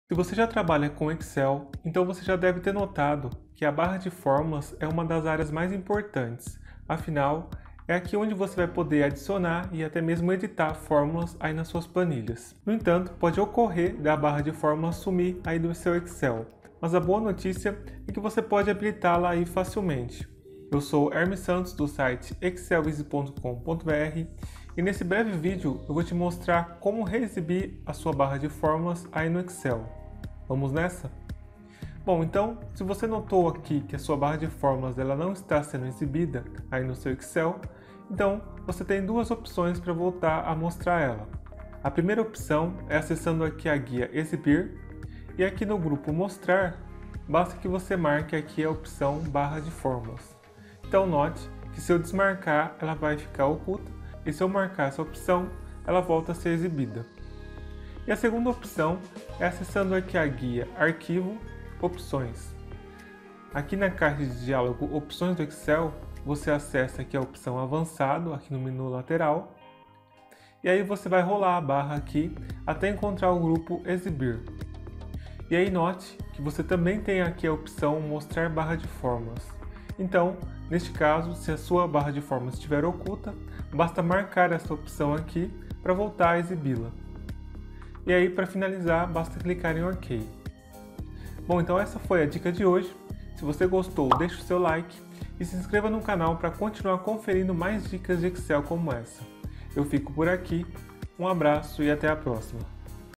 Barra de fórmulas do Excel Sumiu: Como Resolver?
A Barra de fórmulas do Excel Sumiu? Então neste vídeo você vai aprender como voltar a exibi-la em suas planilhas.

Pode-se dizer que a barra de formulas é uma das áreas mais importantes do Excel, afinal, é através dela que podemos inserir e editar fórmulas em nossas planilhas.
No entanto, muitos usuários de Excel acabam se assustando quando a barra de fórmulas desaparece de suas planilhas. E a boa notícia é que existem duas maneiras muito simples de voltar a exibir a barra de fórmulas no Excel.

Confira os passos mostrados no vídeo e veja como é super fácil!
Gostou do vídeo? Então compartilhe para que outras pessoas também possam aprender a reexibir a barra de fórmulas do Excel.